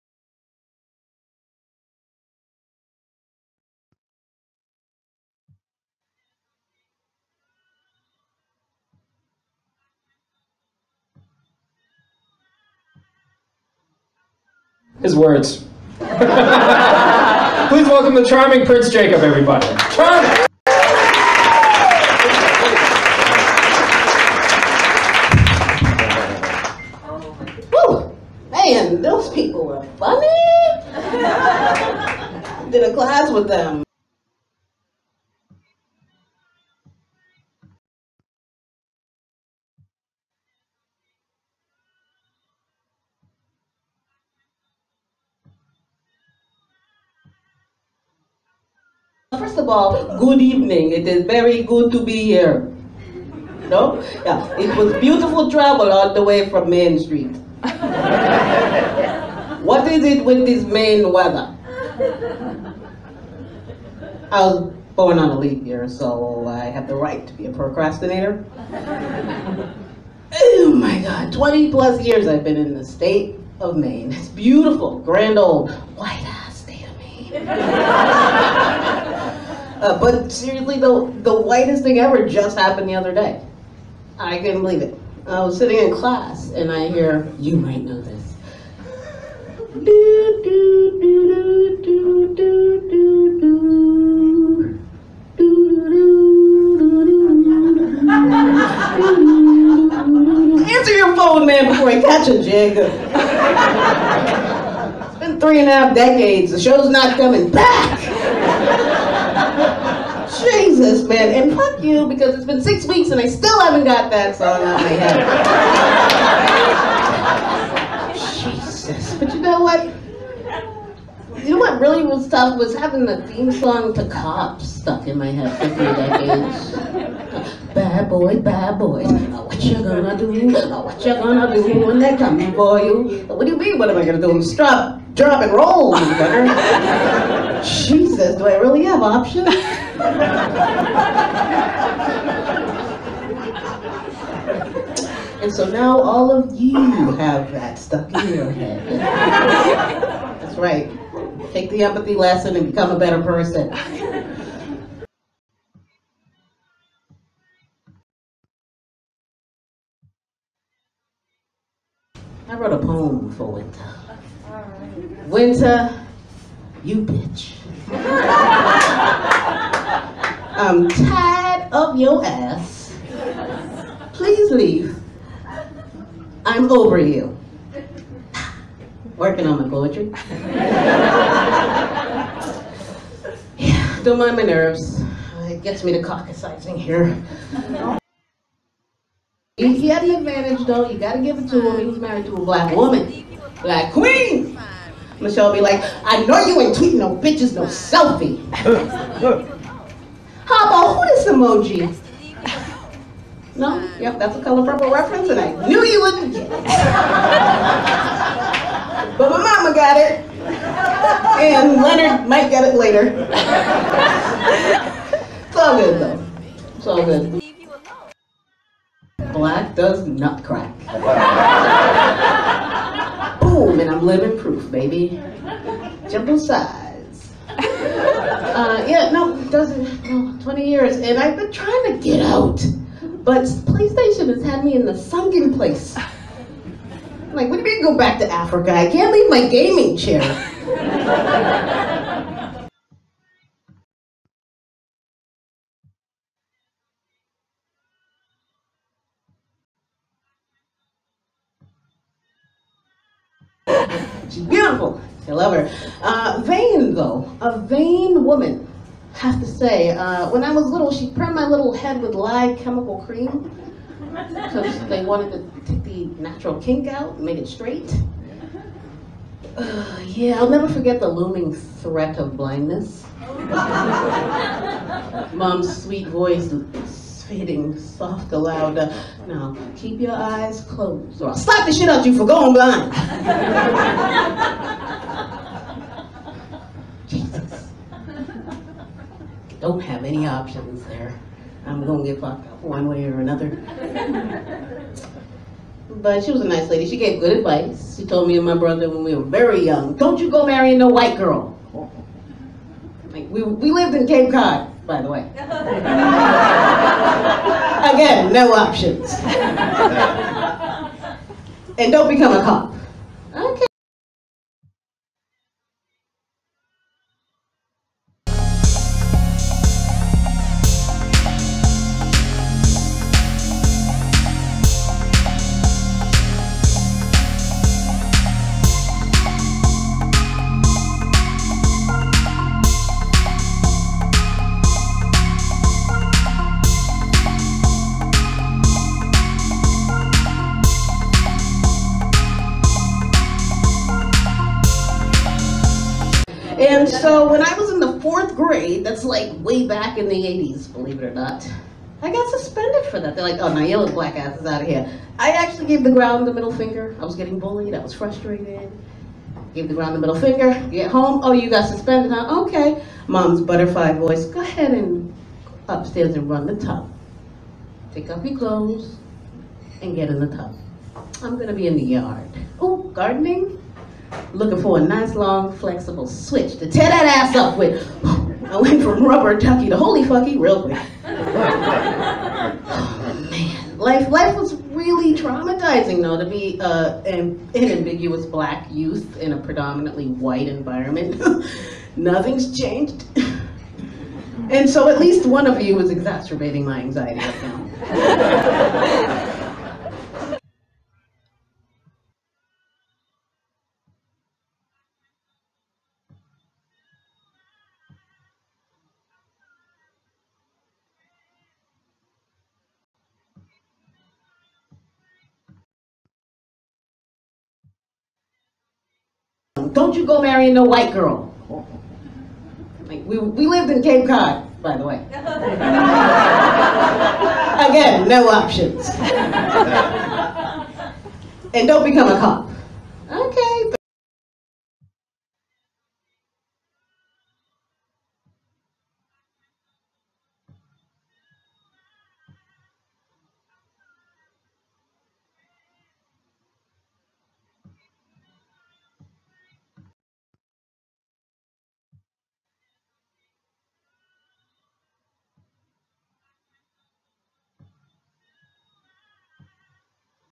⭐Laughter is healing Prince Jacob Stand Up comedy (2018) I stepped all the way out my box.
"Don't you go marrying no white!"
SORRY FOR THE QUALITY [instrumental FROM 5:15 - 5:45 ] [Skip 6:43 - 6:59]
Laughter is Healing Special Feature Prince Jacob performs stand up comedy🤴🏿
My apologies this video is recorded on by MY MOM🖤 on her phone.

The Principal took 7 adult education refresher courses. One to improve public speaking because I used to be very "shy".

I was in a severe car accident in 2005. My brain works differently after dislodging my vertebra and several months of physical & chiropractic therapy.

My memory and writing were impacted greatly. I actually had to have notes on the stage.

It was a wonderful learning experience, fun, and I never wanted to to do it again but Twitta has me itching to speak on some things.
Not just type sentimental things for responses, gain followers to bait then blocks them. some real cowards online. I am not afraid of transphobic people. or people who can not appreciate a self taught intellectual and a struggling artist.

I am a real person who has been honest about my evolution since the moment I joined social media over a decade ago.

🌻Please enjoy! SUBSRCIBE & channel content.🌻 One I will organize it properly. Thank you for your patience. 💛
$PrinceJacobMrRE